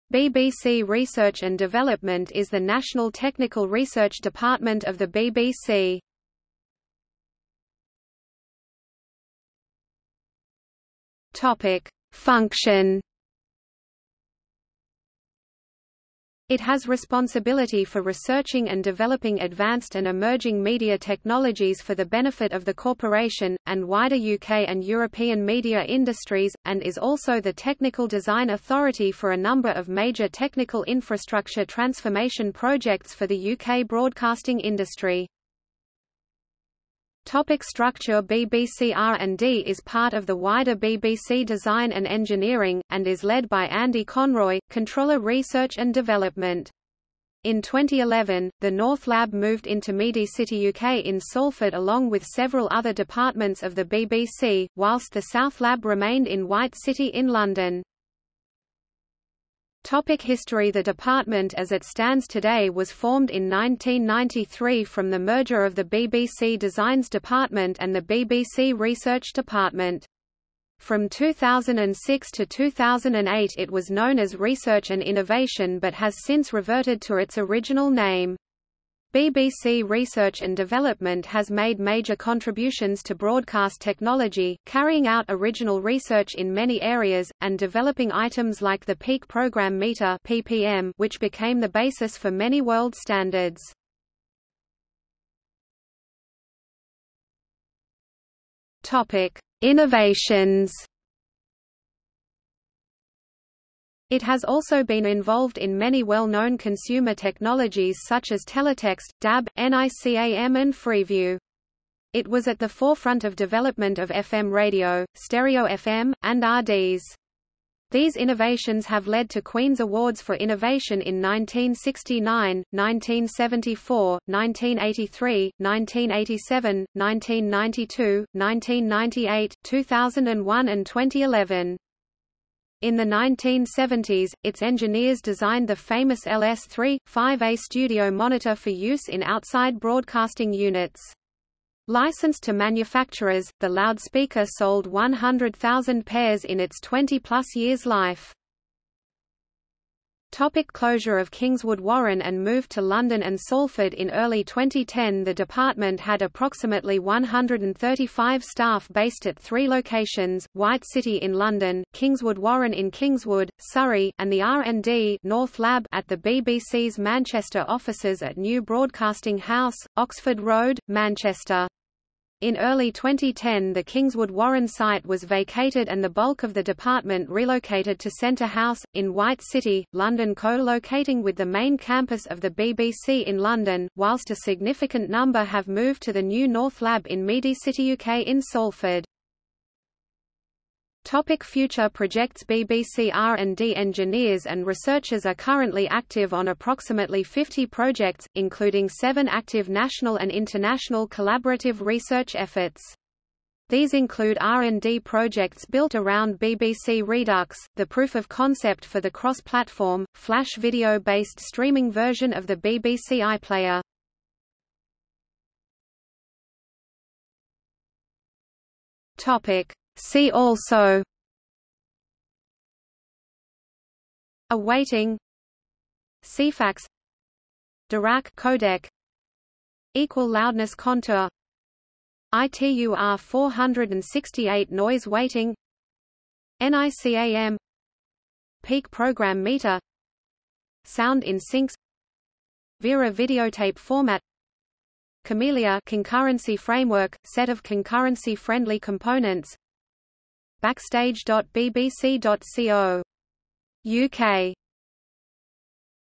BBC Research Department | Wikipedia audio article
00:00:07 1 Function
00:00:37 2 Structure
00:00:59 3 History
00:01:35 3.1 Innovations
00:02:30 3.2 Closure of Kingswood Warren and move to London and Salford
00:03:10 4 Future projects
00:03:35 5 See also

- Socrates

SUMMARY
BBC Research & Development is the national technical research department of the BBC.